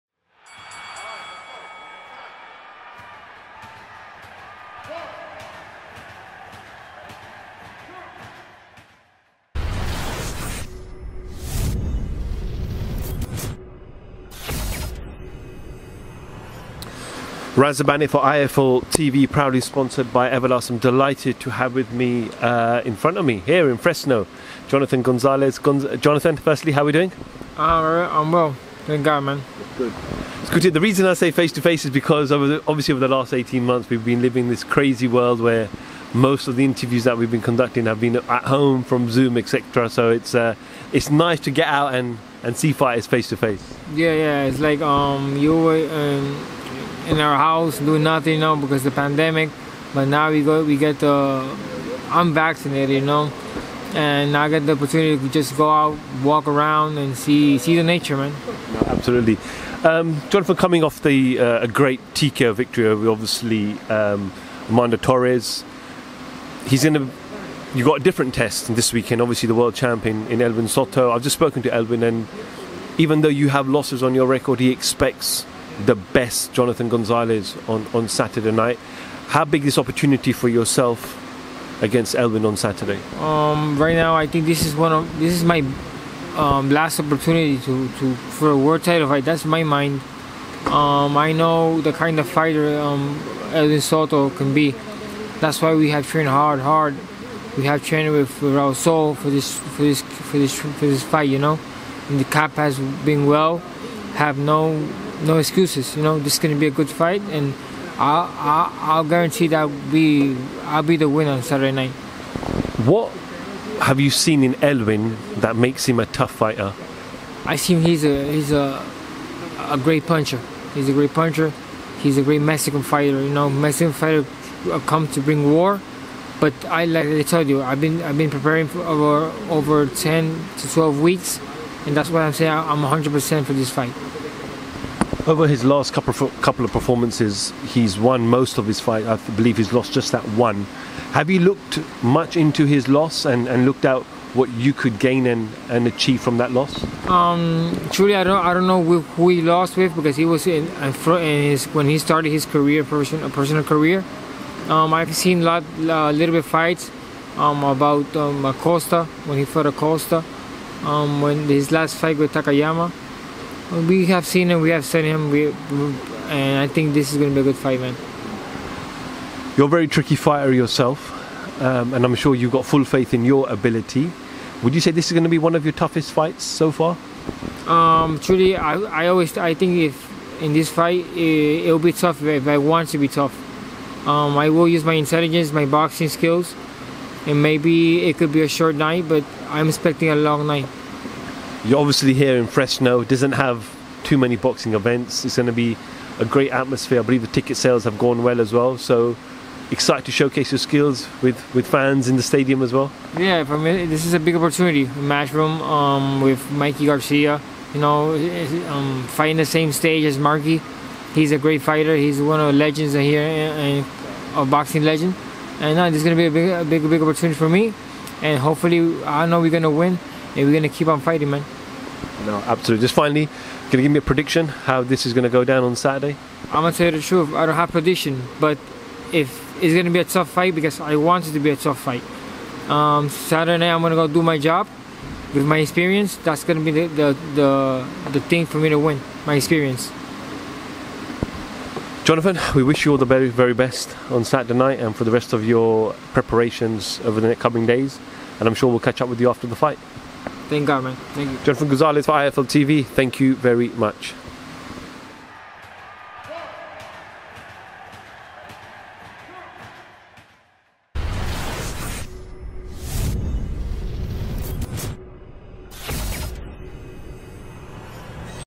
'THIS IS MY LAST WORLD TITLE CHANCE' -JONATHAN GONZALEZ ON ELWIN SOTO FIGHT / SAYS 'I WILL BEAT HIM'
*INTERVIEW BY @RAZA_BAN*